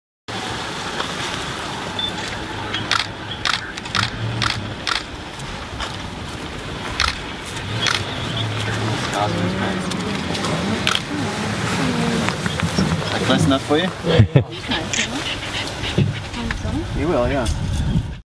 Scarface Walks By
This was one of the more memorable faces we saw on our tour with Harkness Safaris in Botswana.  I nicknamed this fellow "Scarface" because he had obviously been through a few fights.  He was heading to a nearby watering hole for a drink - our jeep happened to be in his path.  In typical lion-like fashion, he sauntered past us, barely giving us a glance as he walked by.  But for us, it was exciting to see this fellow up close.